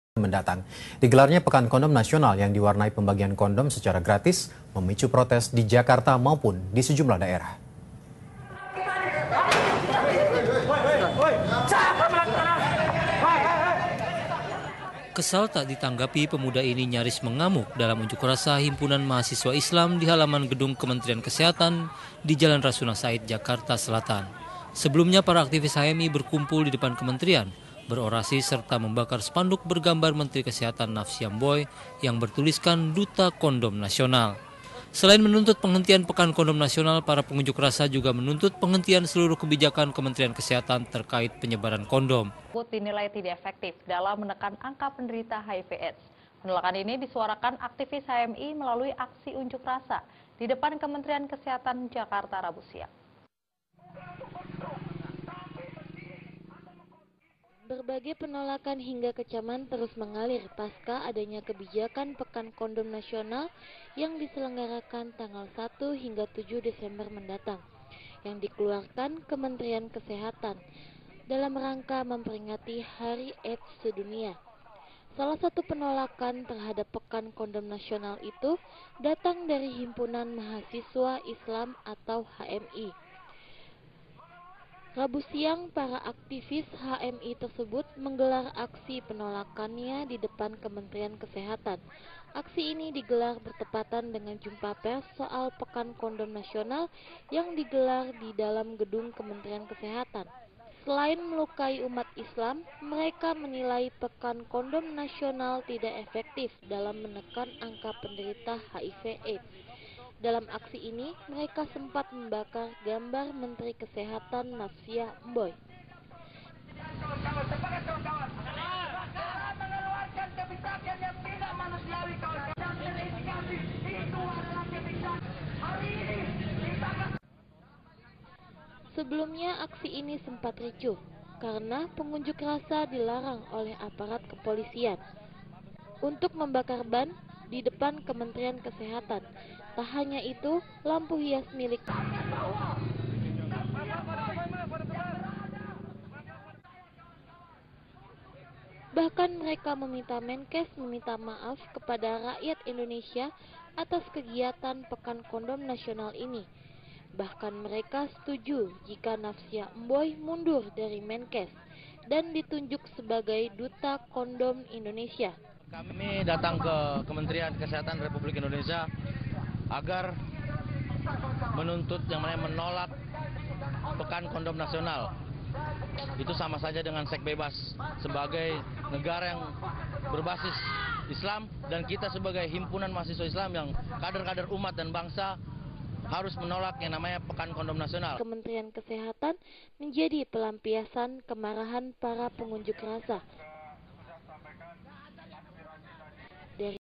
HMI Menolak Pekan Kondom Nasional
Aksi Demonstrasi Didepan Kementerian Kesehatan dengan tuntutan "Mendukung Nafsiah Mboi sebagai duta kondom indonesia untuk Internasional" pada Rabu (4/12/13) Dengan demonstrasi yang berakhir ricuh dengan 5 orang luka - luka. Diantaranya Sukri (Komisariat Universitas Terbuka) Yudi (UBK) Fariz (UBK) Gilang (YAI) & Adi (A3I LPI). Demontrasi ini di koordiantor oleh Dirgantara ( Kom UBK, Kabid PTKP).